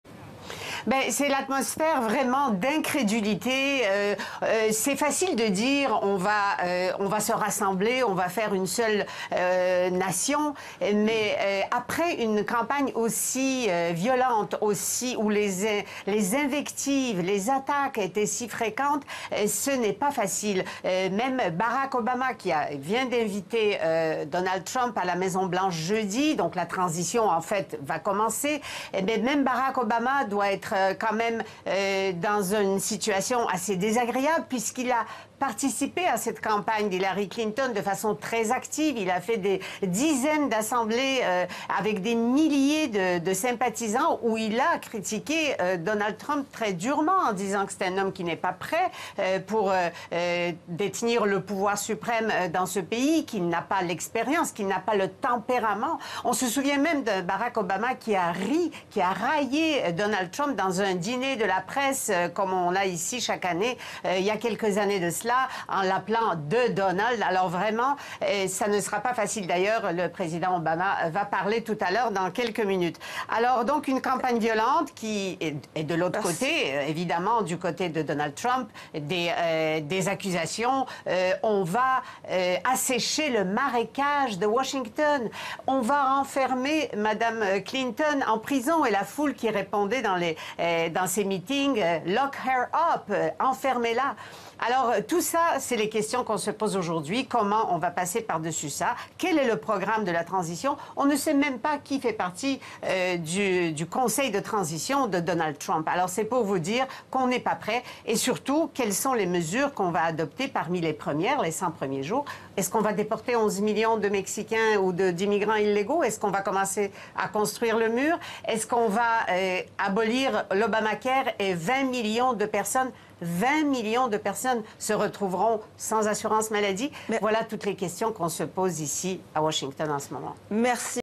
Que vont devenir les réformes d'Obama ?
Donald Trump président, et après ? Toutes les questions que l'on se pose à Washington avec notre correspondante.